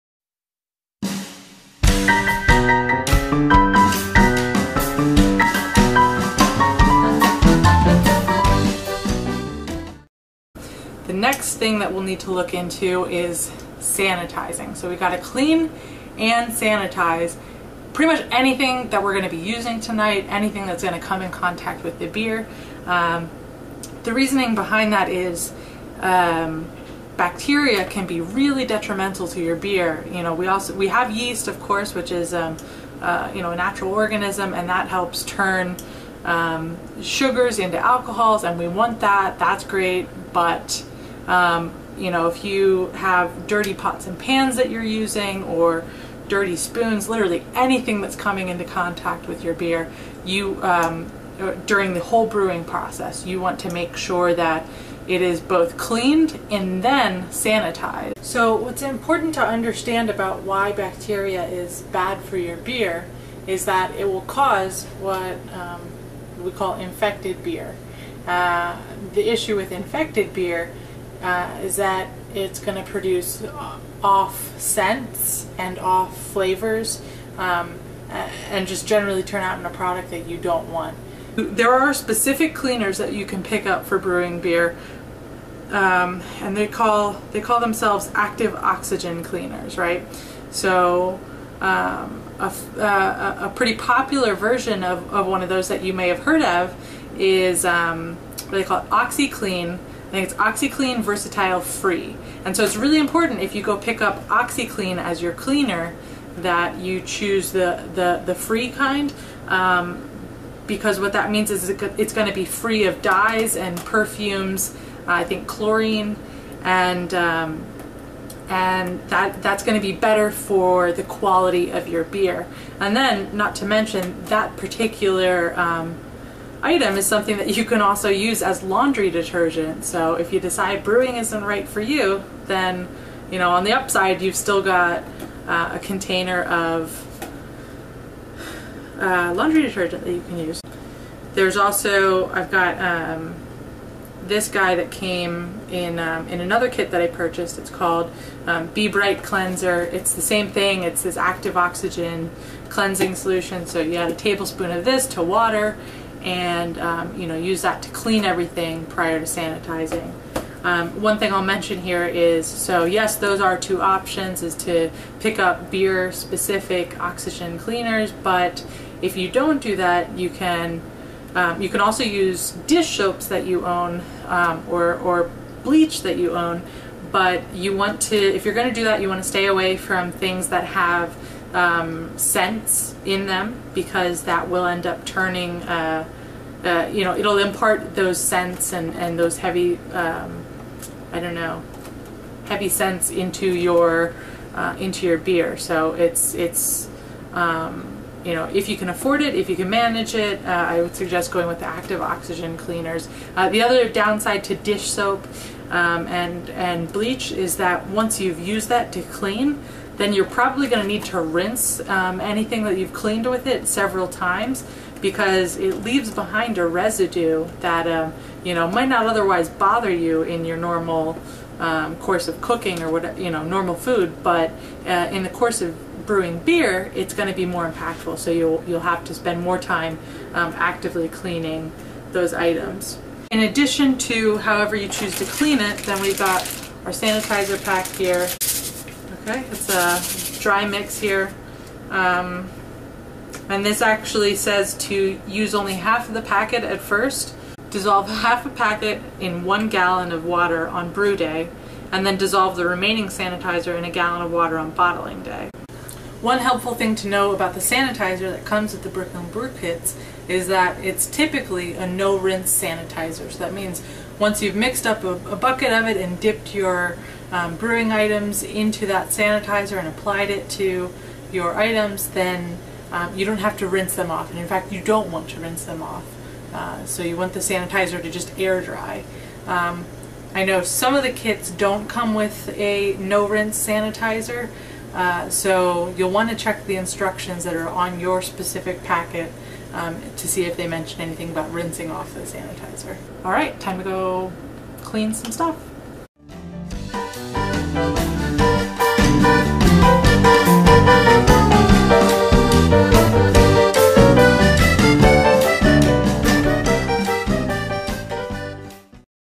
Brooklyn Brewshop Kit - Step 1 - Cleaning and Sanitizing
This is 100% the most important part of the brewing process or all will be for naught! This covers how to clean and sanitize your Brooklyn Brew Shop kit. Sanitize immediately before using your brewing equipment.